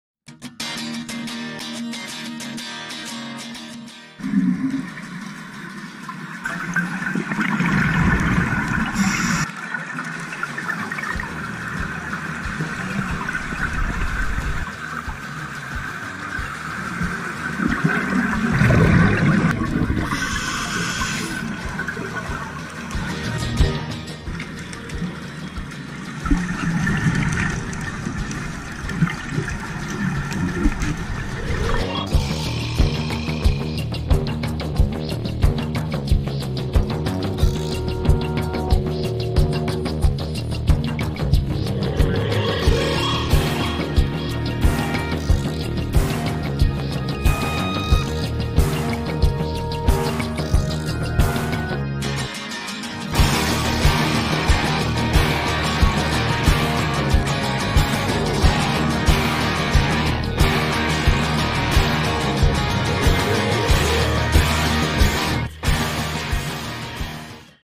[Scuba Dive] [Brazil | Fernando de Noronha] Pontal do Norte
Dive Date: October 24th, 2018
Dive Site: Pontal do Norte
City: Fernando de Noronha | Pernambuco
Country: Brazil
Boat: Mergulhão-X
Dive Operator: Águas Claras